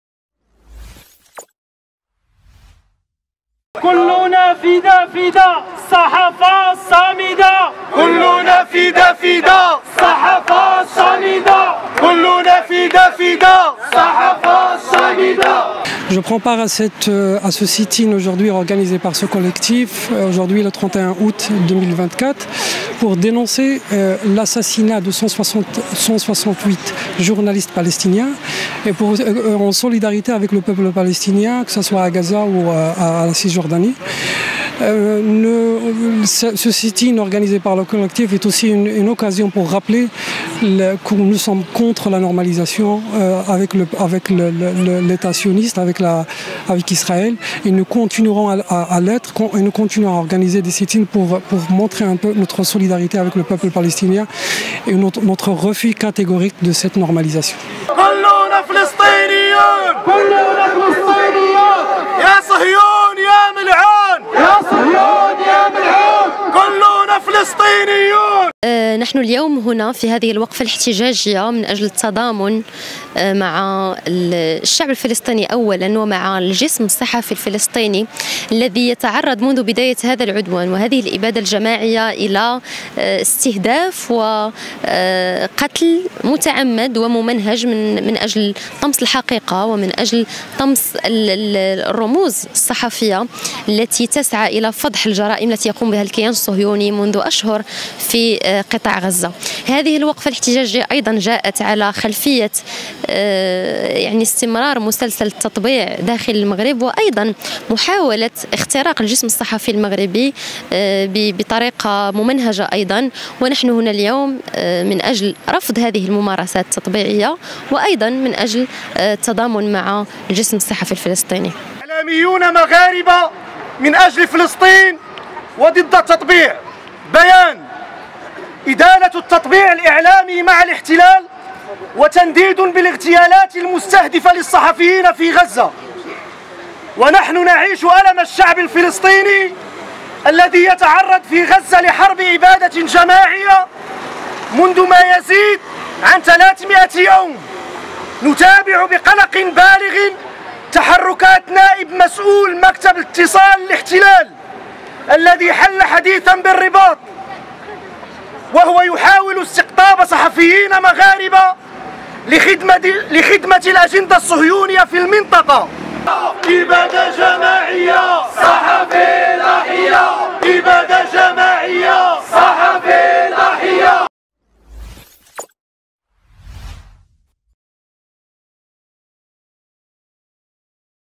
À Rabat, des journalistes marocains mobilisés pour la Palestine
📌 Le collectif « Journalistes marocains pour la Palestine et contre la normalisation avec l’occupation sioniste » a organisé un sit-in devant le  siège du Parlement à Rabat le samedi 30 août 2024, à 19h. Cette manifestation visait à dénoncer les assassinats de journalistes à Gaza, dont le nombre a dépassé les 170 depuis le 7 octobre 2024, et à exprimer leur opposition à la normalisation médiatique au Maroc. 🔎 Reportage.